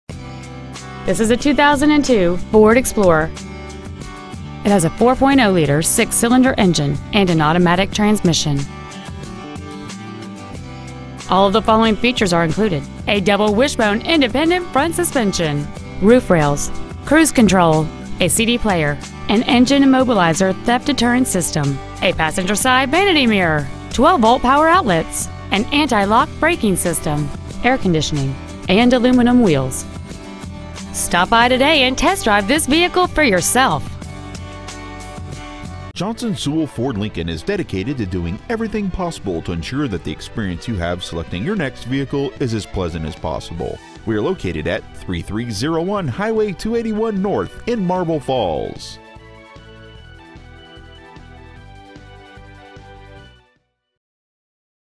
2002 FORD EXPLORER SPORT Marble Falls TX
Year : 2002
 Make : FORD
 Model : EXPLORER SPORT
 Engine : 4.0L V6 12V MPFI SOHC
 Trans . : AUTOMATIC
 Exterior : WHITE
 Miles : 167,456
 Interior : BLACK
 Stock : PE1083A